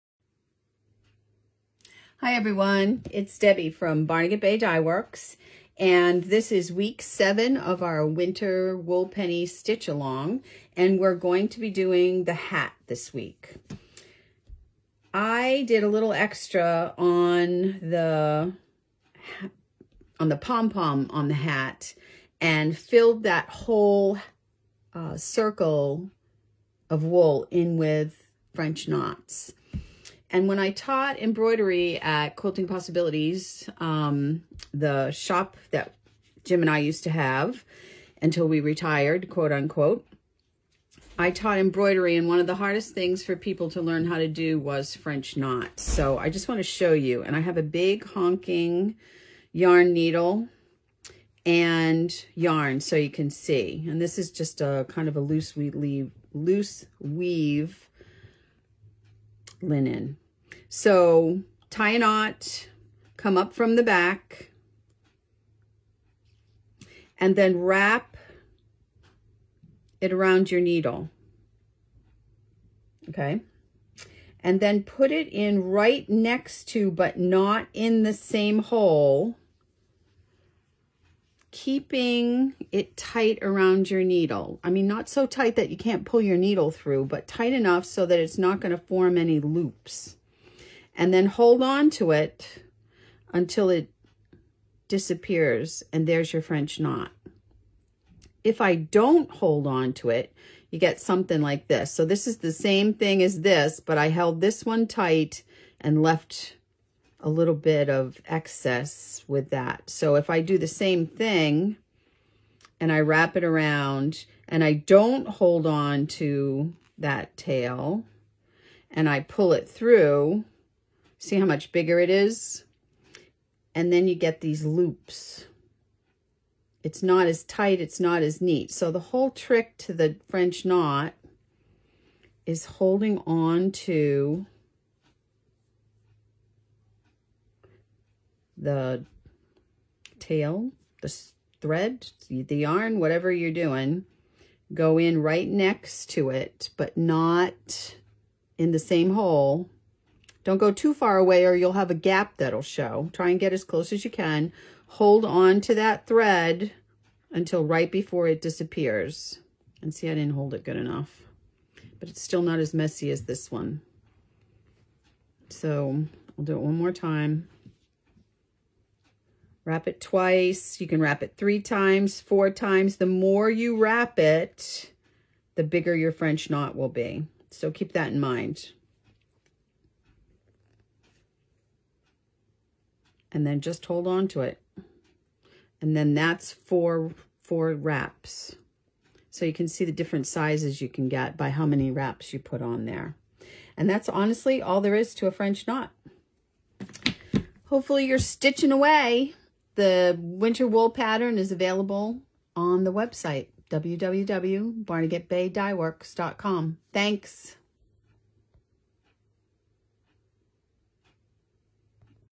French Knots How To
French Knots - you either love them or don't but French Knots give texture and dimension to wool applique, hand embroidery and any other hand stitching.  Here is a tip on how to stitch French Knots without any big loops.